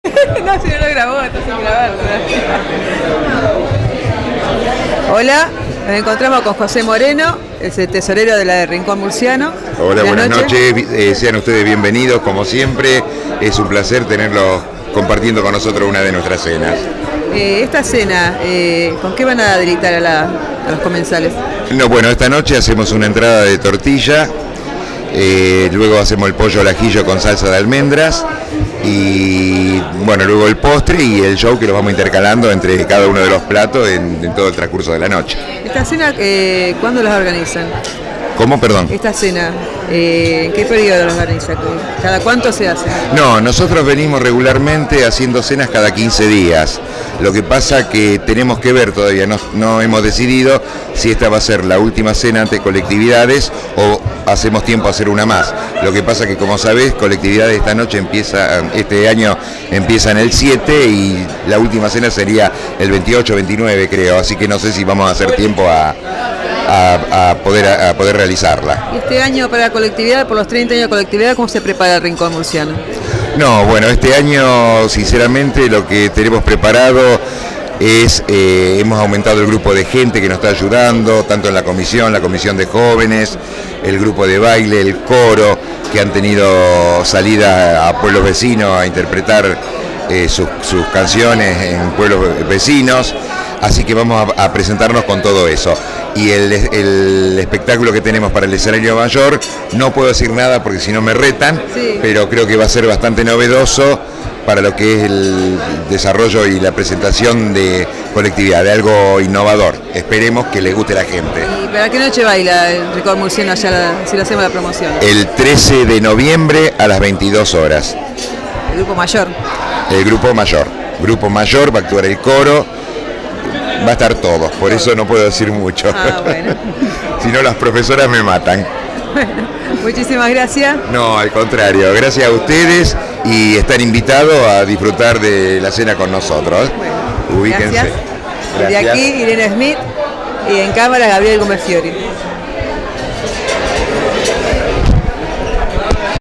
Jose del Rincón Murciano de Rosario
José, Tesorero del Rincón Murciano de Rosario, comenta algunas actividades realizadas durante 2014, sobre La Noche de Colectividades y los preparativos para el 30º aniversario del Encuentro de Colectividades